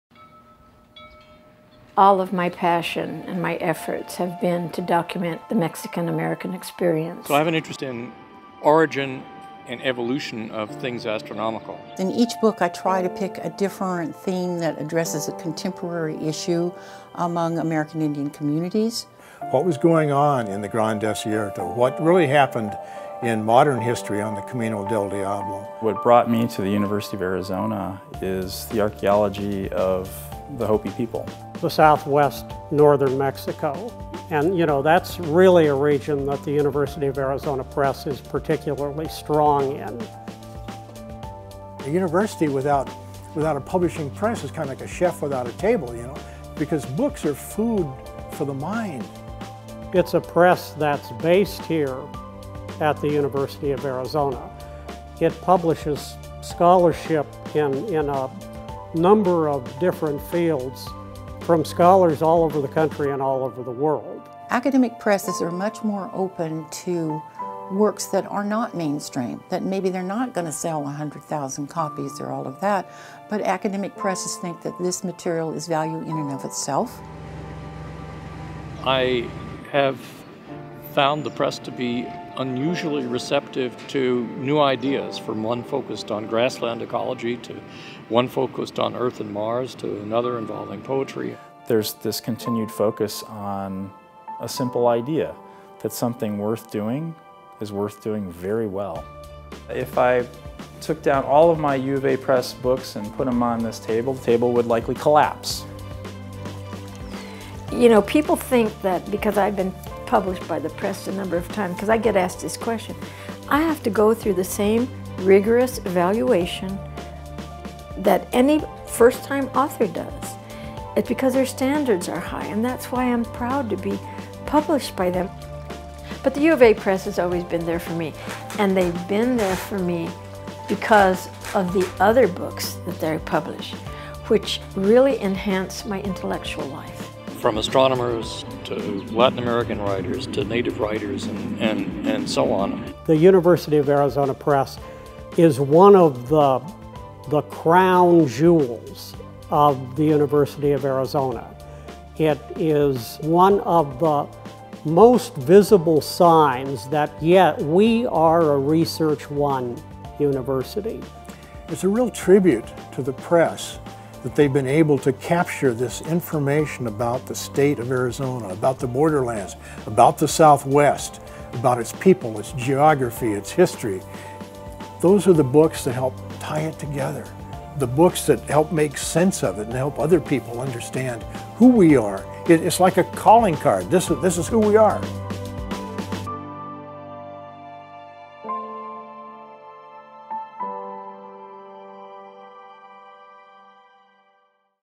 University of Arizona Press | Who We Are
The University of Arizona Press is the premier publisher of academic, regional, and literary works in the state of Arizona. We disseminate ideas and knowledge of lasting value that enrich understanding, inspire curiosity, and enlighten readers. We advance the University of Arizona’s mission by connecting scholarship and creative expression to readers worldwide.